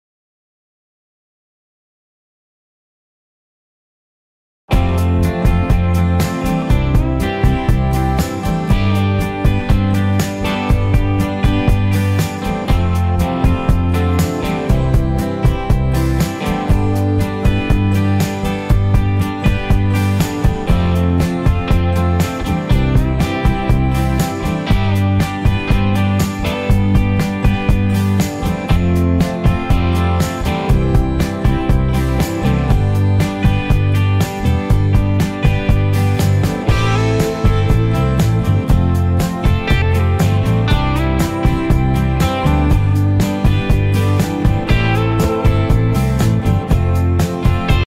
2.4 MARLA FLAT FOR SALE AT DALAZAK ROAD PESHAWAR
In main The City mall
Dalazak Road
Near Aman Chowk
Flat no 607
Down payment
On possession10%
Half yearly

Monthly Installment 45080
2BedRooms
Attach Bath
Kitchen
Tv loungh
Good location
Secure Area